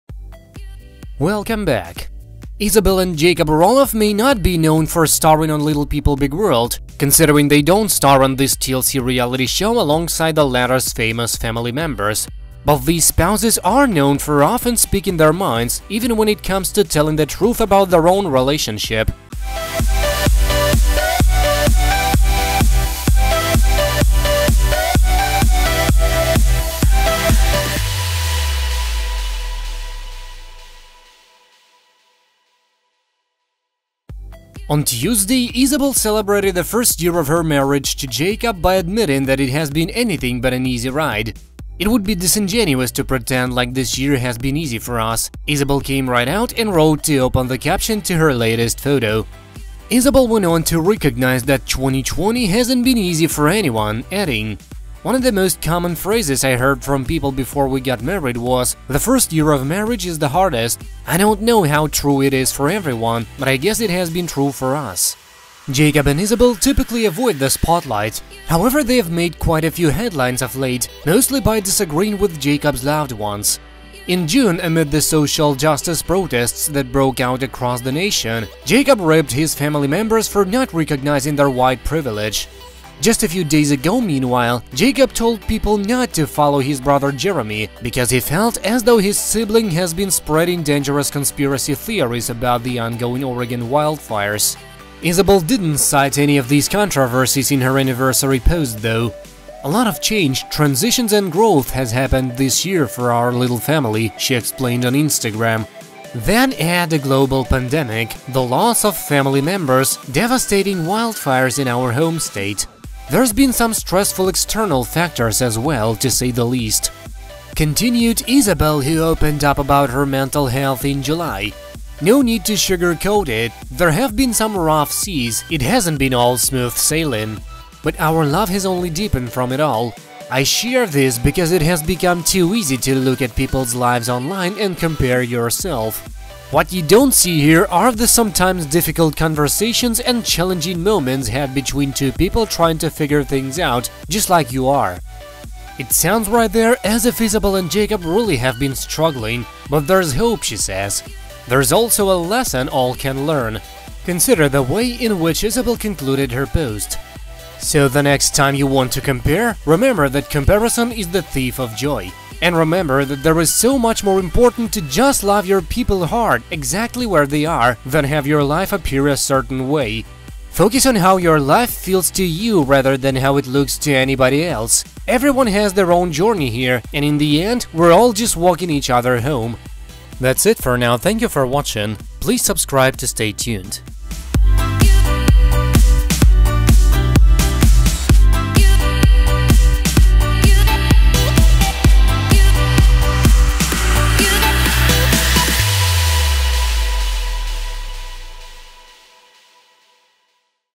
Isabel Roloff Gets Real About Marriage: It's Been Very Rough
Isabel Roloff and Jacob Roloff may not be known for starring on Little People, Big World...

... considering they don't star on this TLC reality show alongside the latter's famous family members.

But these spouses are known for often speaking their minds, even when it comes to telling the truth about their own realtionship.

Isabel Roloff Wedding Pic
On Tuesday, Isabel celebrated the first year of her marriage to Jacob by admitting that it has been anything but an easy ride.

"It would be disingenuous to pretend like this year has been easy for us," Isabel came right out and wrote to open the caption to the photo above.

Isabel went on to recognize that 2020 hasn't been easy for anyone, adding:

"One of the most common phrases I heard from people before we got married was “the first year of marriage is the hardest”. I don’t know how true it is for everyone-- but I guess it has been true for us."

Isabel Roloff and Jacob
Jacob and Isabel typically avoid the spotlight.

However, they've made quite a few headlines of late -- mostly by disagreeing with Jacob's loved ones.

In June, amid the social justice protests that broke out across the nation, Jacob ripped his family members for not recognizing their white privilege.

Just a few days ago, meanwhile, Jacob told people NOT to follow his brother, Jeremy, because he felt as though his sibling has been spreading dangerous conspiracy theories about the ongoing Oregon wildfires.

Jacob Roloff and His Better Half
Isabel didn't cite any of these controversies in her anniversary post, though.

"A lot of change, transitions, and growth has happened this year for our little family," she explained on Instagram.

"Then add a global pandemic, the loss of family members, devastating wildfires in our home state.

"There’s been some stressful external factors as well, to say the least."

Jacob and Wife and Mom
Continued Isabel, who opened up about her mental health in July:

"No need to sugar coat it, there have been some rough seas; it hasn’t been all smooth sailing. But our love has only deepened from it all.

"I share this because it has become too easy to look at people’s lives online and compare yourself.

"What you don’t see here are the sometimes difficult conversations and challenging moments had between two people trying to figure things out, just like you are."

Jacob and Isabel Together
It sounds right there as if Isabel and Jacob really have been struggling.

But there's hope, she says.

There's also a lesson all can learn.

A Big Kiss for Isabel Rock
Consider the way in which Isabel concluded her post:

So the next time you want to compare, remember that comparison is the thief of joy.

And remember that it’s so much more important to just love your people hard, exactly where they are, than have your life appear a certain way.

Focus on how your life feels to you rather than how it looks to anybody else.

Everyone has their own journey here, and in the end, “we’re all just walking each other home.”